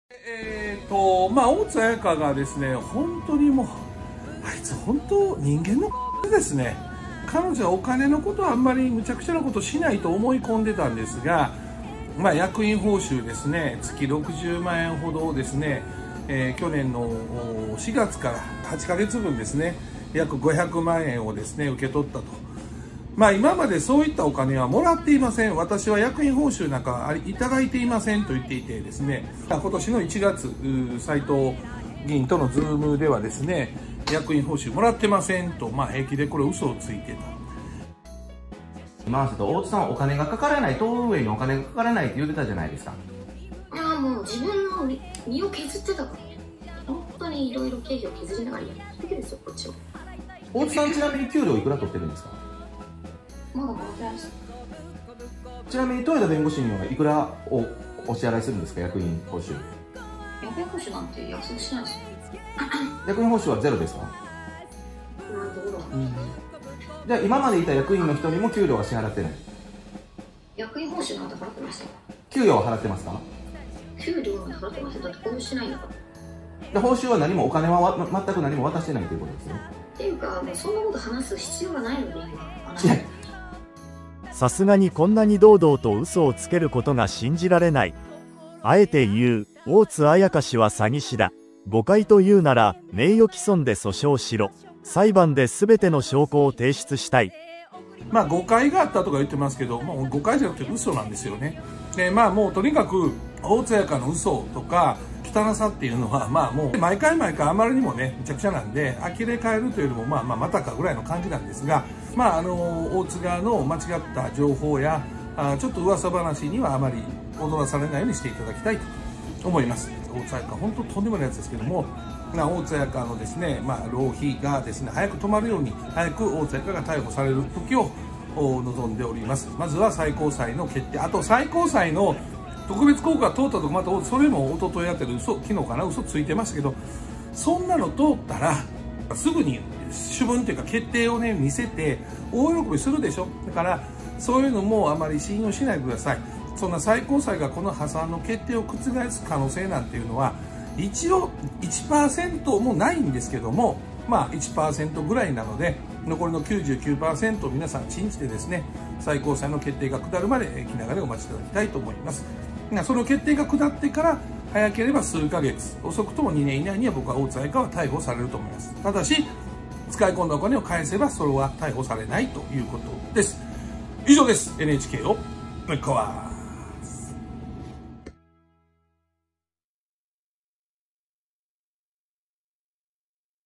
【大炎上】これは許されない…!!「報酬は貰っていません」とあれほど偉そうに断言していた大津綾香氏が実は数百万円もの大金を受け取っていたことが判明！これが国政政党の党首のやることか！
【元動画】

NHKをぶっ壊すTV　　(入荷待ちです…)

─ 当チャンネル最新人気動画 ─